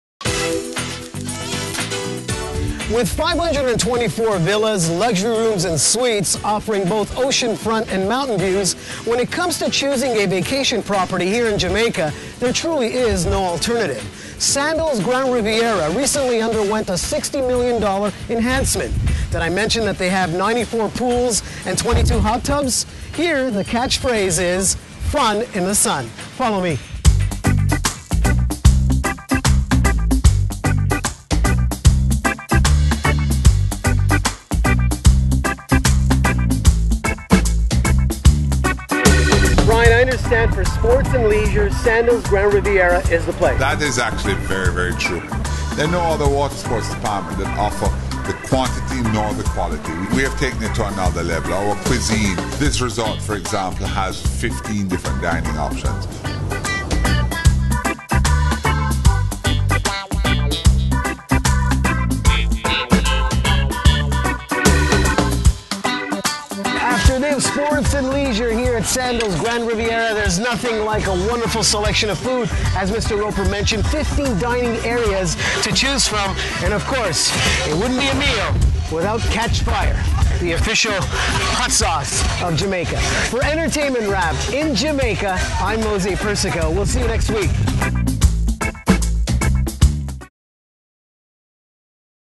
Sandals Grande Riviera
Entertainment Wrap's Mosé Persico visits the Sandals resort Grande Riviera in Ocho Rios Jamaica